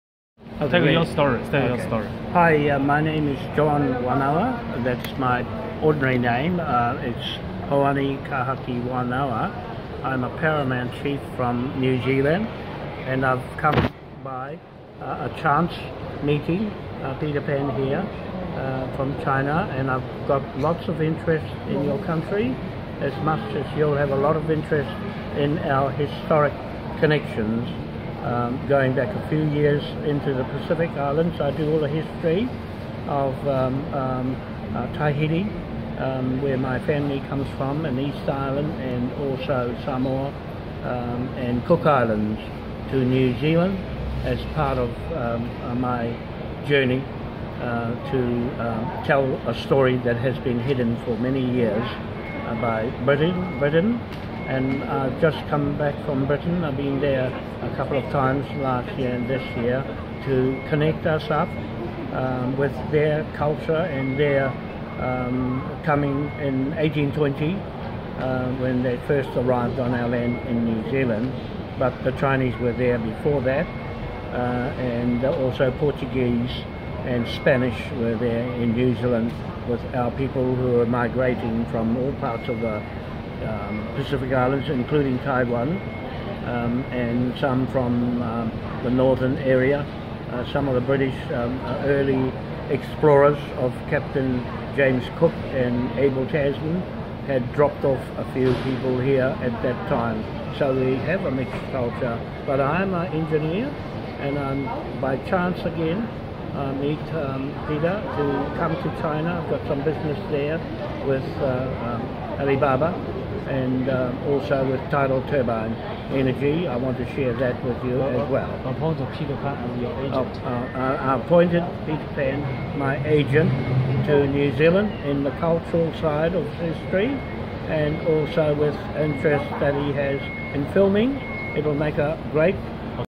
Peter Pan in Hong Kong Ambassador for me in New Zealand My Story
Published Saturday 21 December 2019

Peter Pan made this video at Hong Kong Airport He wants to act as Ambassador to New Zealand from China to tell my story to Chinese people